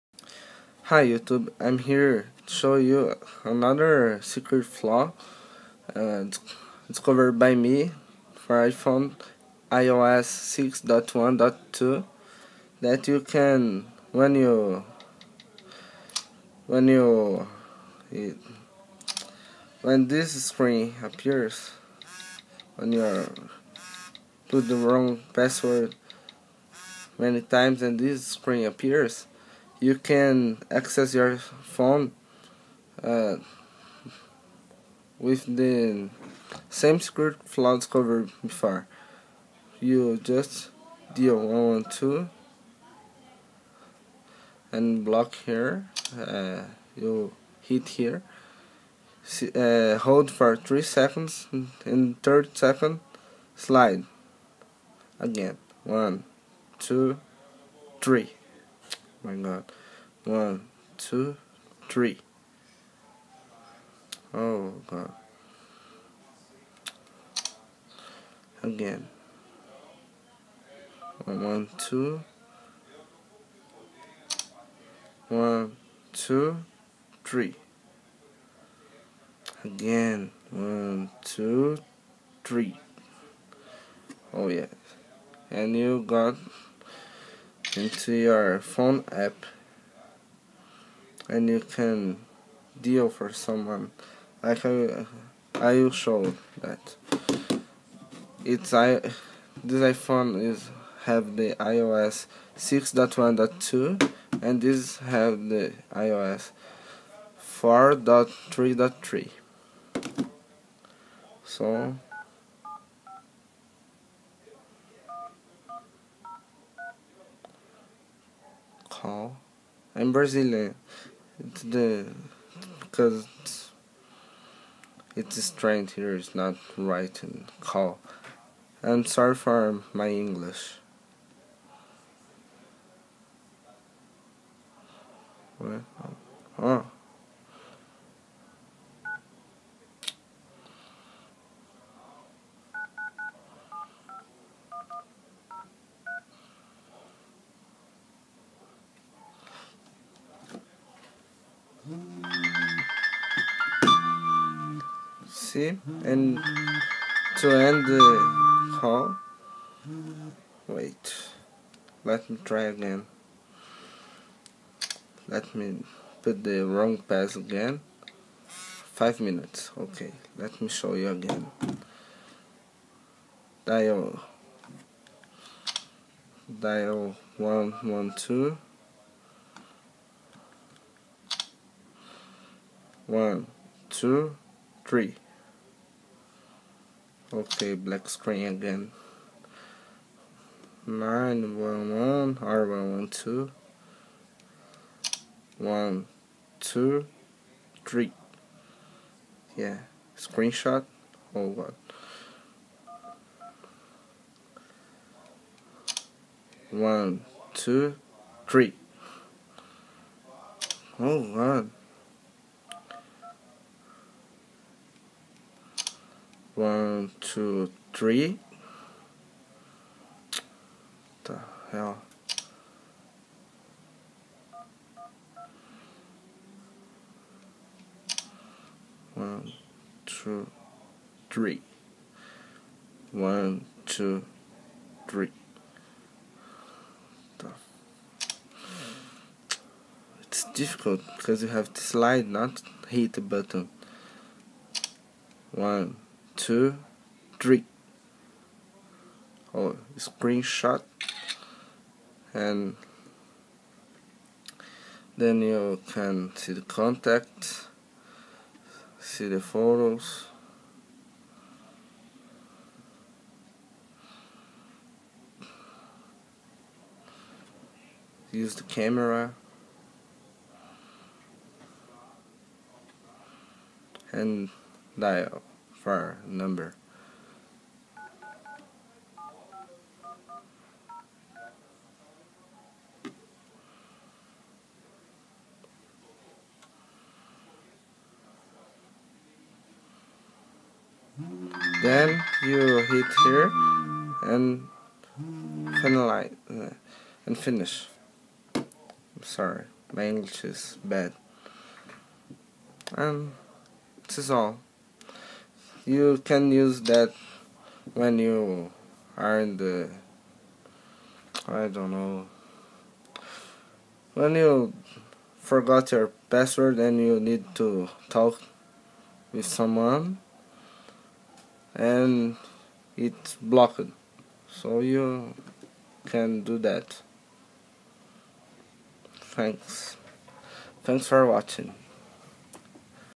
English - Bypass "Iphone is Disabled" Screen and access Phone App
I'm here to show you a new security flaw discovered by me tha allow you to access your contacts, photos, send mails, send sms, call a number and access voicemail in "Iphone is Disabled" screen.

"Iphone is Disabled" screen appears when you put the wrong passcode several times.